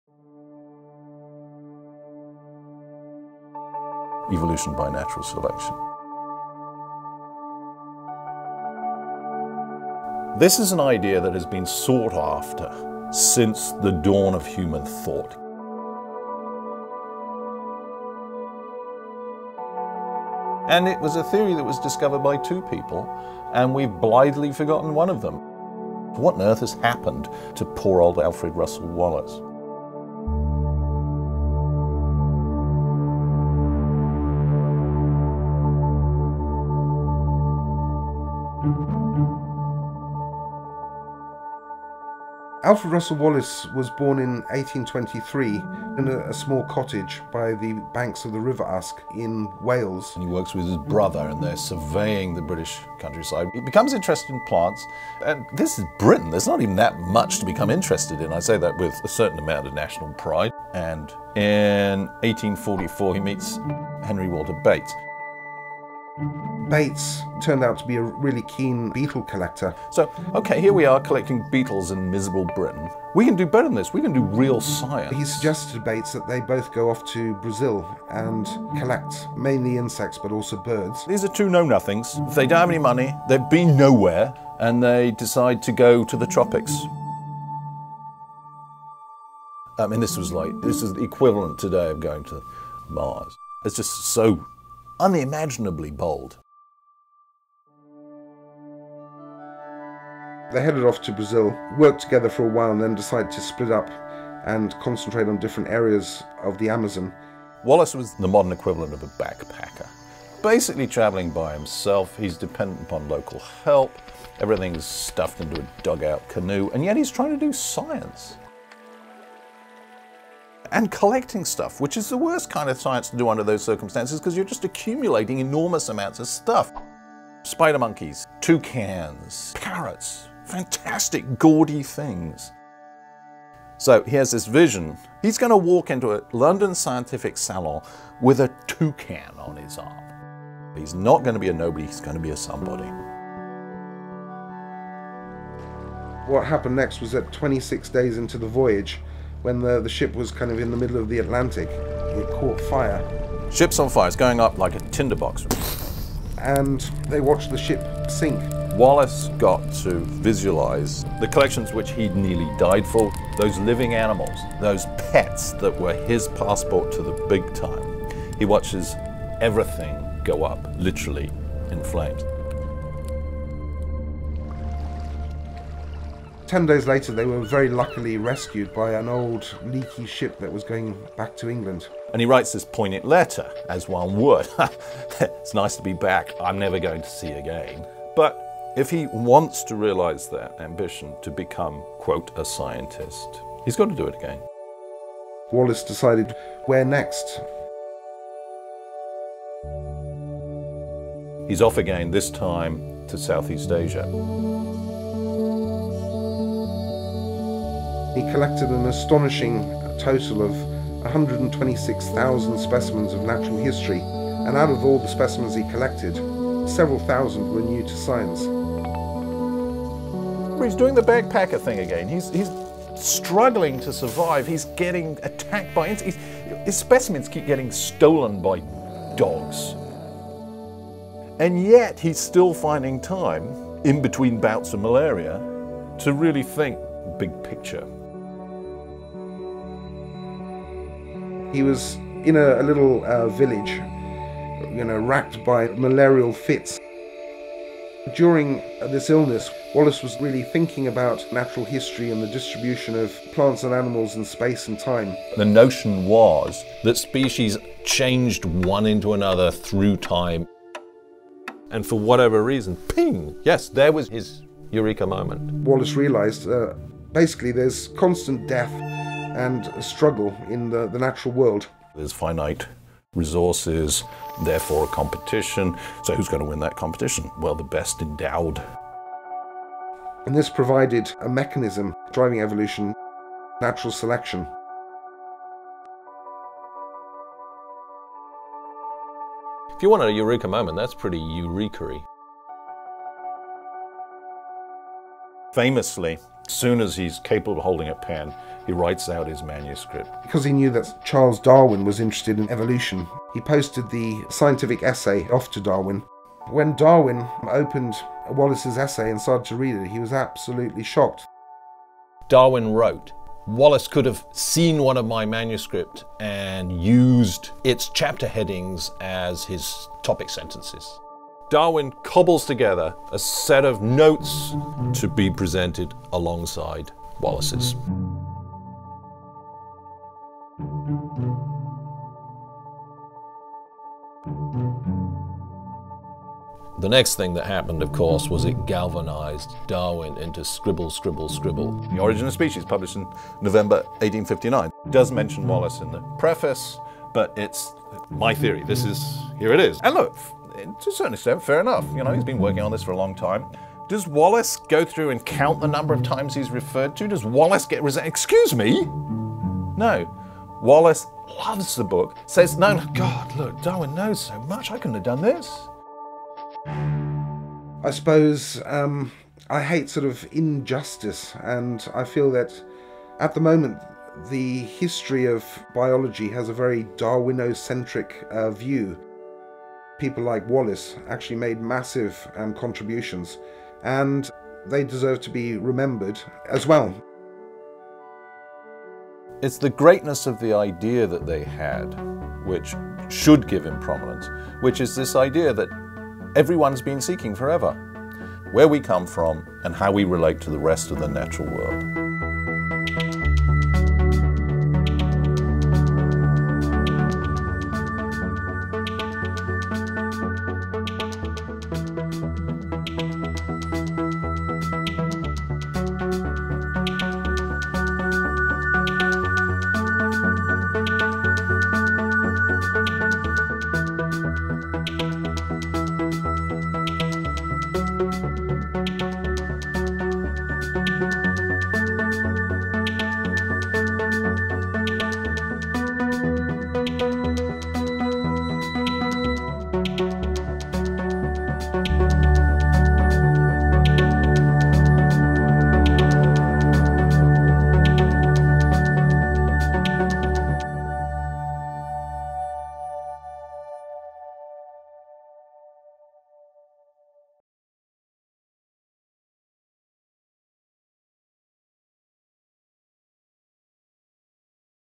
The Life of Alfred Russel Wallace — HHMI BioInteractive Video
Entertain your science classroom with a paper-cut animation video tracking the theory of evolution as formulated by Alfred Russel Wallace. Wallace independently formulated his theory of evolution by natural selection at the same time as Charles Darwin. This fun video animates Wallace’s life, from his boyhood in England to his voyages with Henry Walter Bates and his later adventures in the Malay Archipelago.

Learn about one of the founders of natural selection and the theory of evolution in this video, perfect for high school biology lesson plans. Co-directed by Flora Lichtman and Sharon Shattuck of Sweet Fern Production, this film provides an in-depth and entertaining look at Wallace’s life.

Free classroom resources supporting this short film can be found